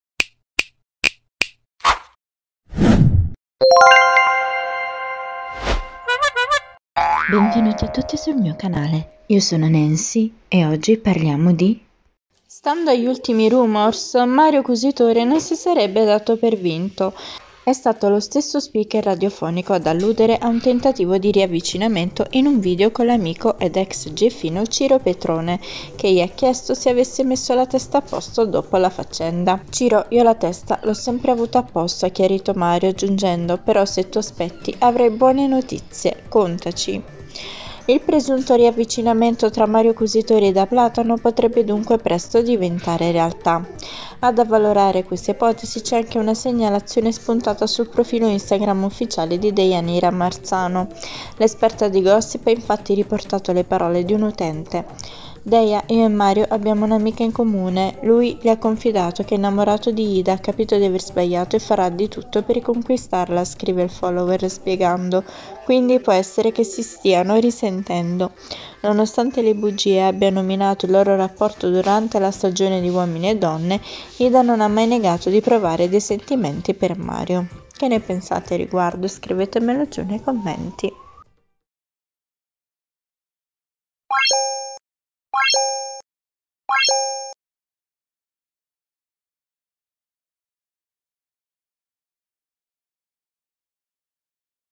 Uomini e Donne, Mario Cusitore non molla Ida Platano: “Buone notizie”, lo zampino di Ciro Petrone
Buone notizie per Mario e Ida? uominiedonne idaplatano mariocusitore Modificato da VideoGuru